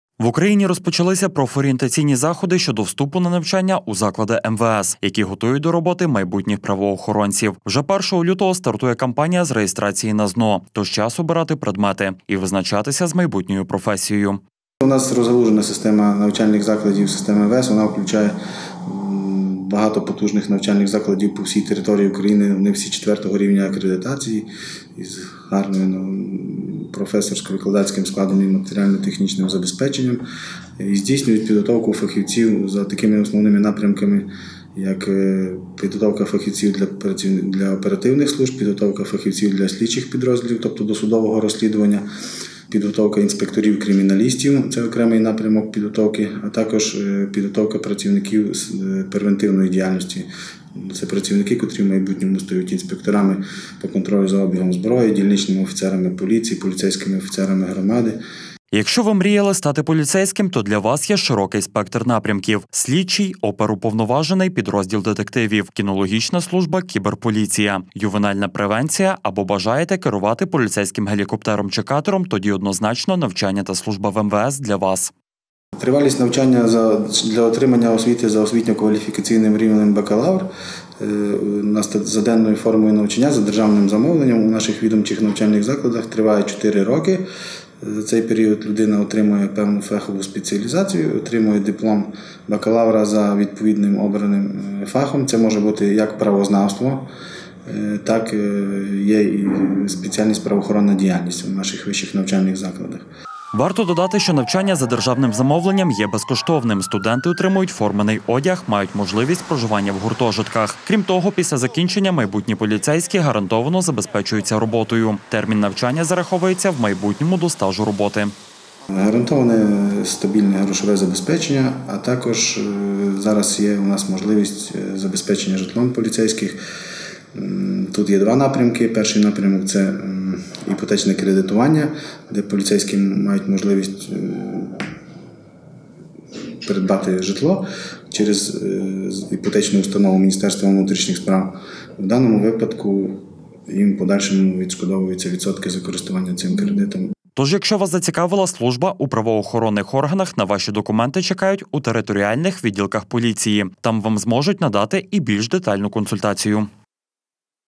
Майбутні правоохоронці. Вступ на навчання
Служити та захищати – таким є основний лозунг українських правоохоронців. Із початку цього року поліцейські розпочали профорієнтаційні заходи стосовно вступу на навчання у закладах МВС. Там готують майбутніх слідчих, кінологів, кіберполіцейських, і не тільки. В Україні розпочалися профорієнтаційні заходи щодо вступу на навчання у заклади МВС, які готують до роботи майбутніх правоохоронців. Вже 1 лютого стартує кампанія з реєстрації на ЗНО, тож час обирати предмети, і визначатися із майбутньою професією. Якщо ви мріяли стати поліцейським, то для вас є широкий спектр напрямків: слідчий, оперуповноважений, підрозділ детективів, кінологічна служба, кіберполіція, ювенальна превенція, або бажаєте керувати поліцейським гелікоптером, чи катером – тоді однозначно навчання та служба в МВС для вас! Варто додати, що навчання за державним замовленням є безкоштовним, студенти отримують формений одяг, мають можливість проживання у гуртожитках,  крім того, після закінчення майбутні поліцейські гарантовано забезпечуються роботою, термін навчання зараховується у майбутньому до стажу роботи. Тож, якщо вас зацікавила служба у правоохоронних органах, на ваші документи чекають у територіальних відділках поліції, там вам зможуть надати і більш детальну консультацію.